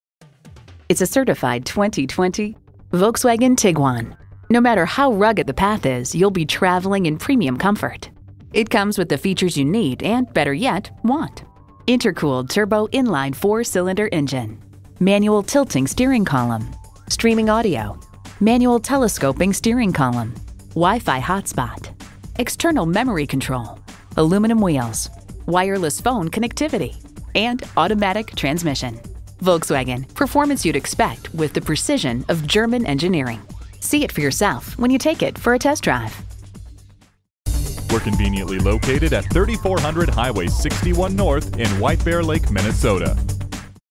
Certified 2020 Volkswagen Tiguan Saint Paul White-Bear-Lake, MN WB0377A - SOLD
Year: 2020
Make: Volkswagen
Model: Tiguan
Trim: S
Engine: 4 Cylinder Engine
Transmission: 8-Speed A/T
Color: WHITE
Interior: Black
Mileage: 18208
Stock #: WB0377A
VIN: 3VV1B7AX9LM114679
Certified with a lifetime powertrain warranty. Economical Front wheel drive. Excellent overall condition.

Address:
3400 Highway 61 North